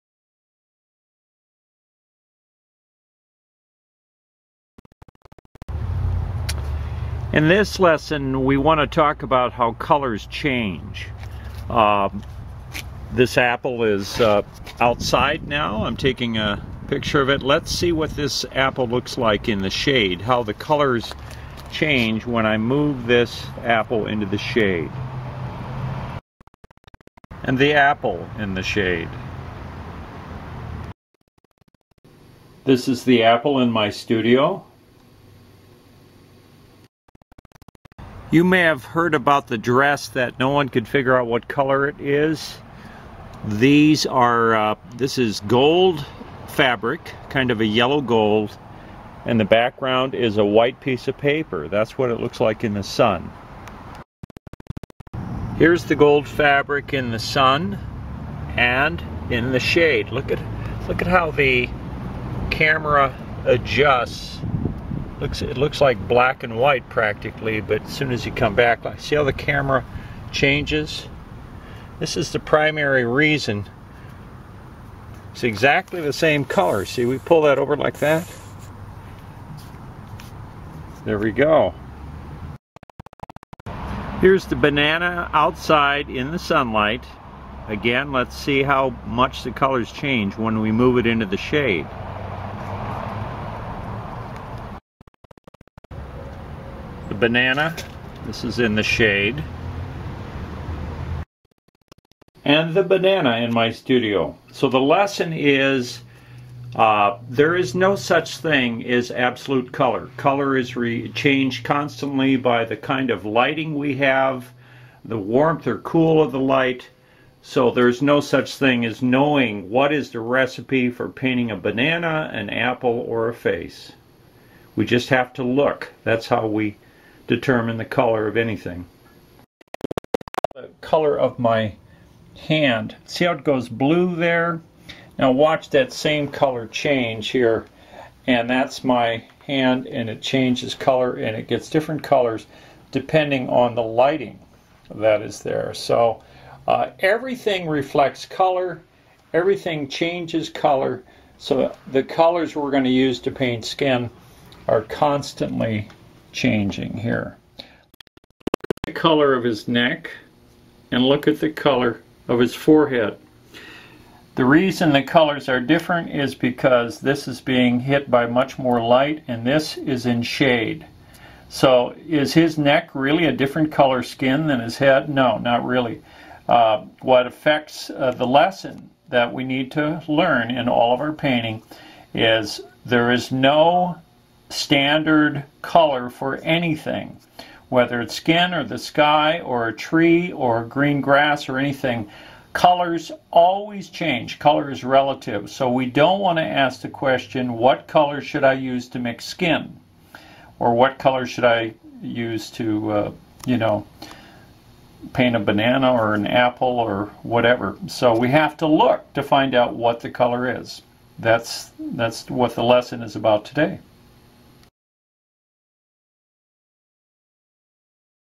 Lesson 21 / How Colors Change / Stan Miller
There is no absolute color.  All color is relative depending on what colors surrounds it (reflected color) and the angle and intensity of available light.  All color turns black when there is no available light.  Consequently, there is no recipe for painting skin, trees, or any subject.  The artist has to observe to determine the correct color.